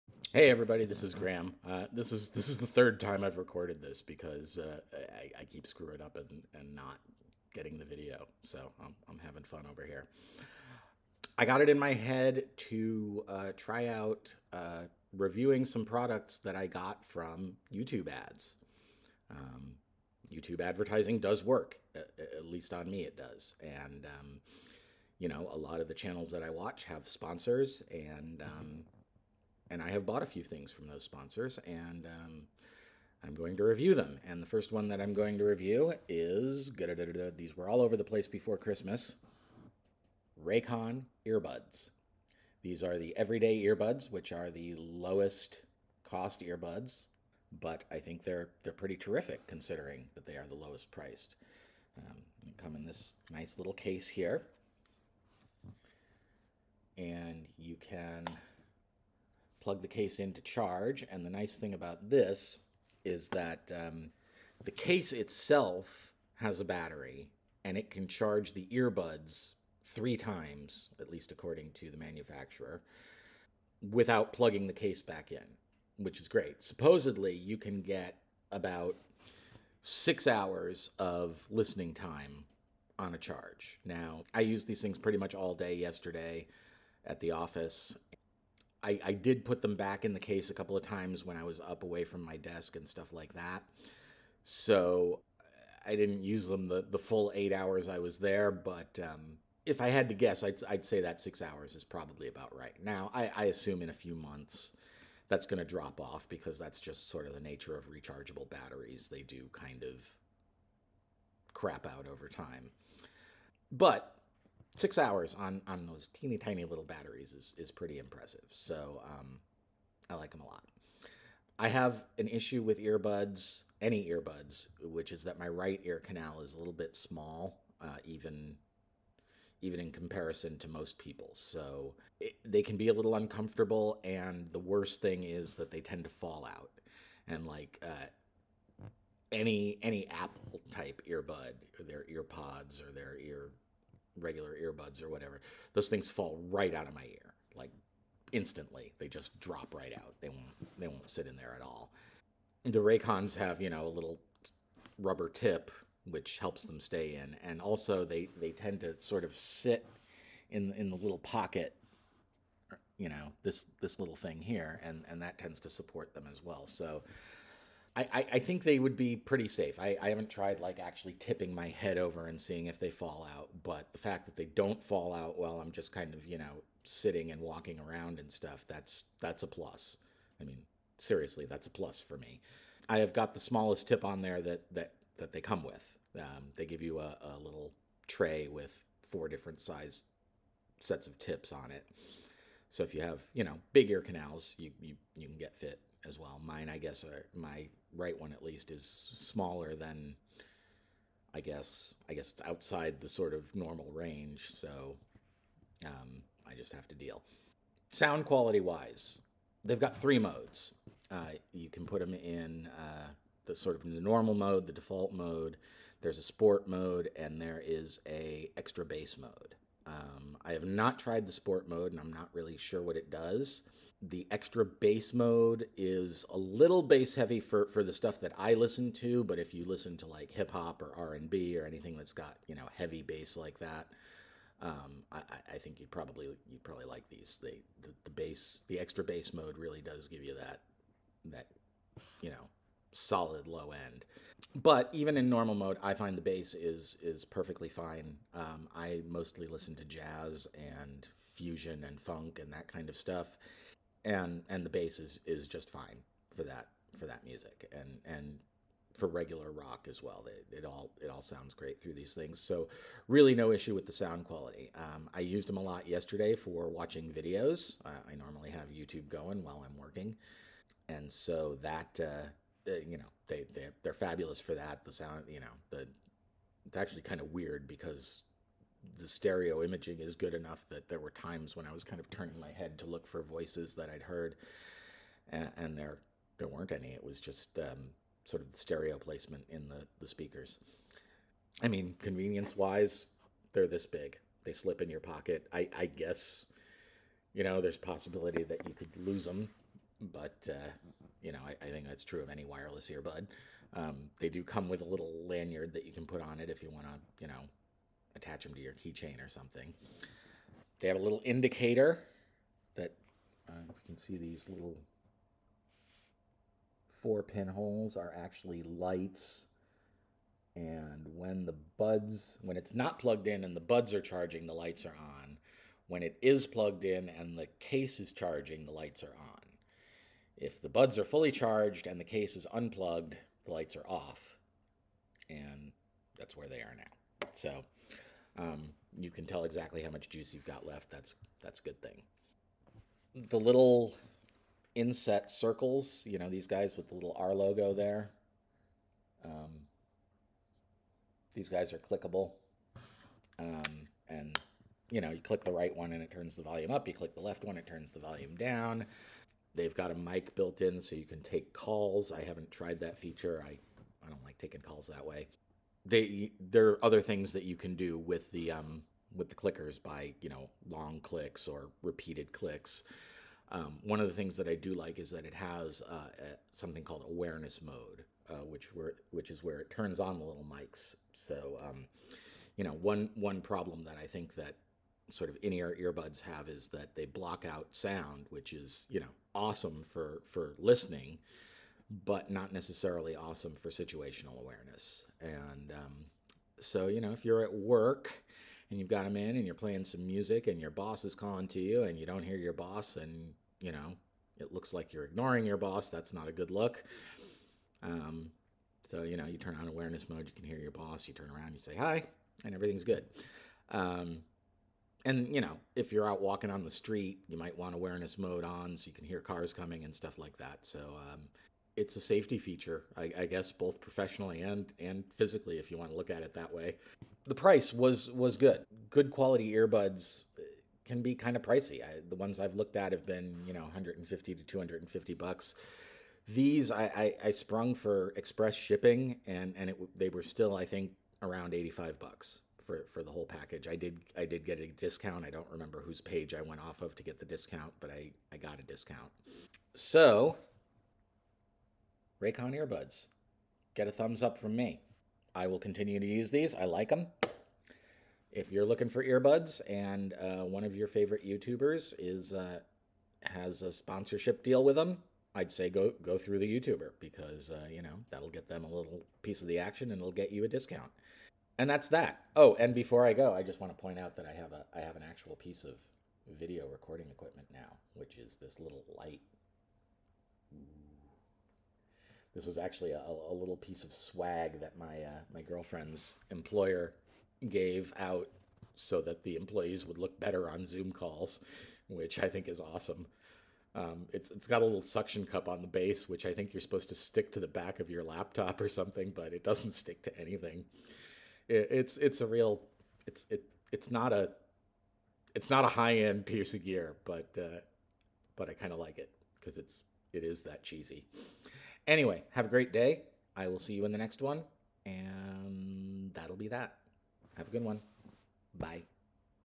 YouTube Sponsor's Product Review: RayCon Earbuds
A whole bunch of channels were advertising these during the holidays. I bought a pair and decided to review them.